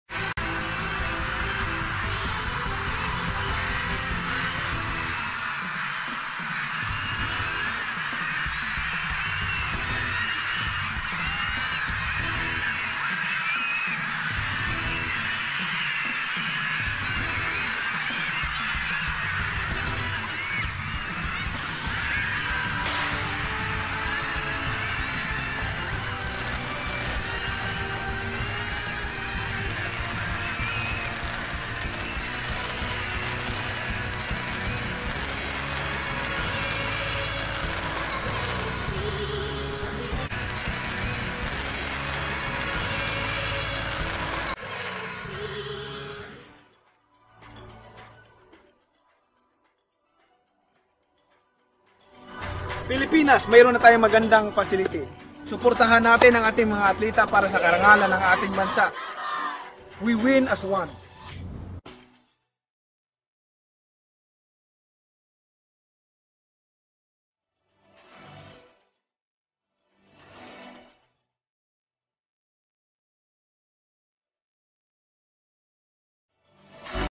WORLD CLASS! SEA Games 2019 Cauldron Lighting by Sen. Manny Pacquiao
World class cauldron lighting by World Boxing Champion and Philippine Senator Manny Pacquiao.